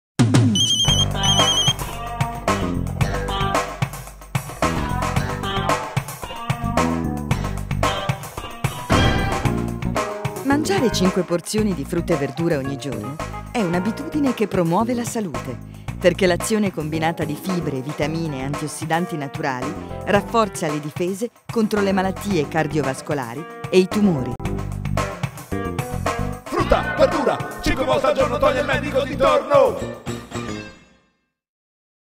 Spot Frutta Verdura
Spot per la campagna “Frutta Verdura: cinque volte al giorno”
Cliente: Regione Emilia-Romagna
Agenzia: Nouvelle
Anno: 2002